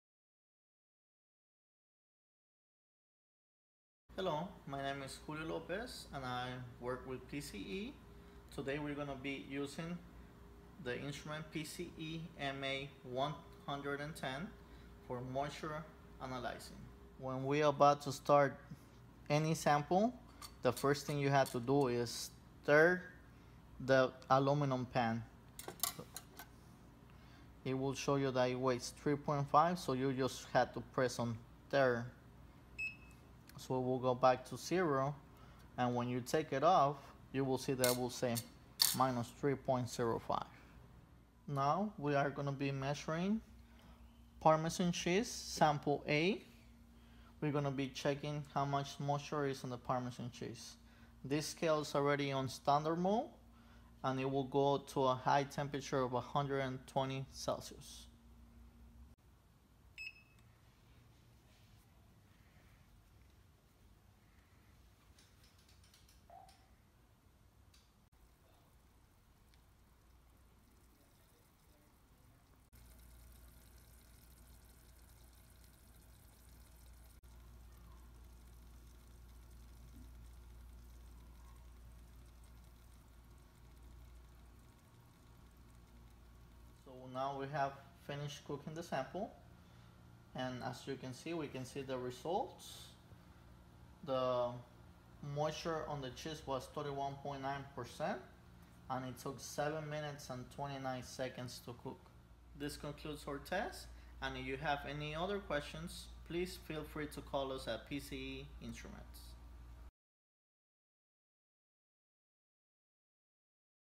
How to Determine Moisture Content in Cheese Using PCE-MA 110 Moisture Balance
Watch as PCE Instruments' Technical Specialist shows you how to use the PCE-MA 110 moisture balance to measure the moisture in a cheese sample. The PCE-MA 110 moisture balance offers a readability of 10 mg / 0.01 g / 0.1% with a testing or weighing capacity of 110 g. For more information about PCE Instruments' moisture balance products,